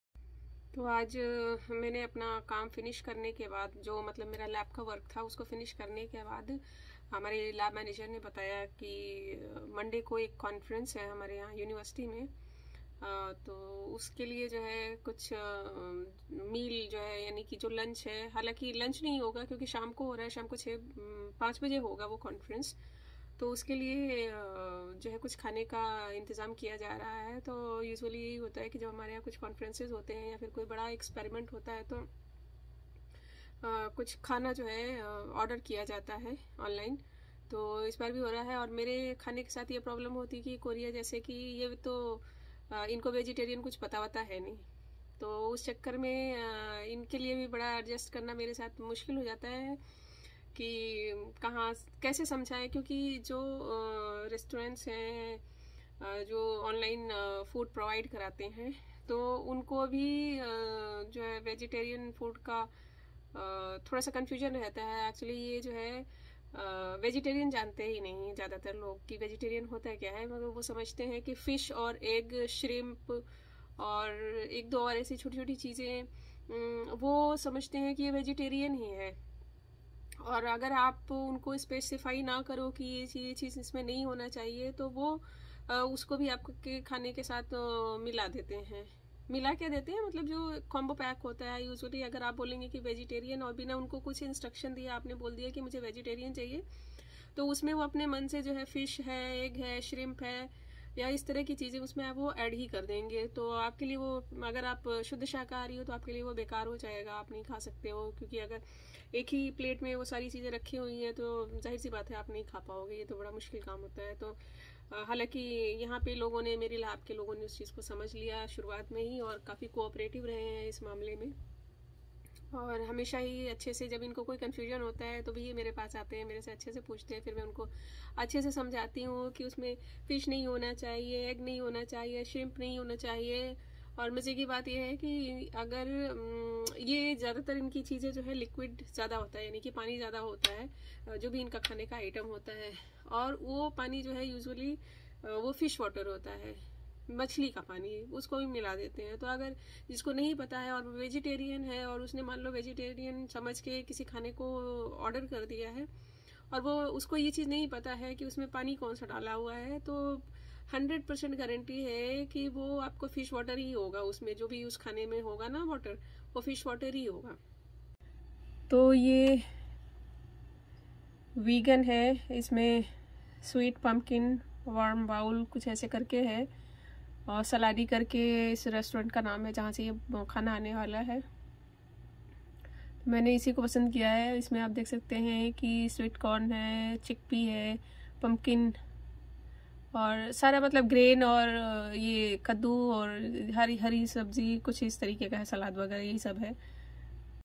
कोरिया में vegetarians survive कर सकते हैं या नहीं | vegetarian food is difficult to find in Korea?🤷
Is vegetarian food available in South Korea?

What do Korean vegetarians eat?
Is vegetarian popular in Korea?

Can pure vegetarian survive in South Korea?
Can a vegetarian eat Tteokbokki?